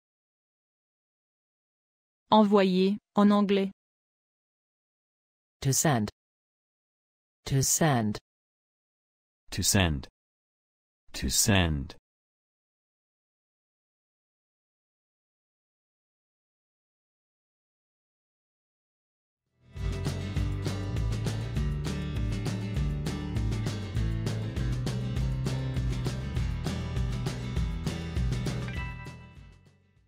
Comment dire envoyer en anglais
Comment dire correctement envoyer en anglais? Ecoute avec nous.
Quelle est la traduction correcte du mot envoyer du français vers l'anglais? Parlez comme les Américains. Aussi, comment écrire correctement le mot to send?